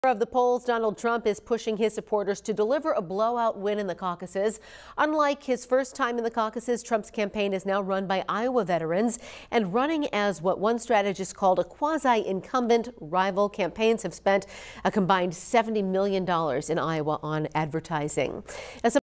With Iowa's caucuses a month away, Trump urges voters to hand him not just a victory, but a blowout
“The margin of victory is very important, it's just very important,” Trump told about 1,000 people attending a Wednesday rally in Coralville.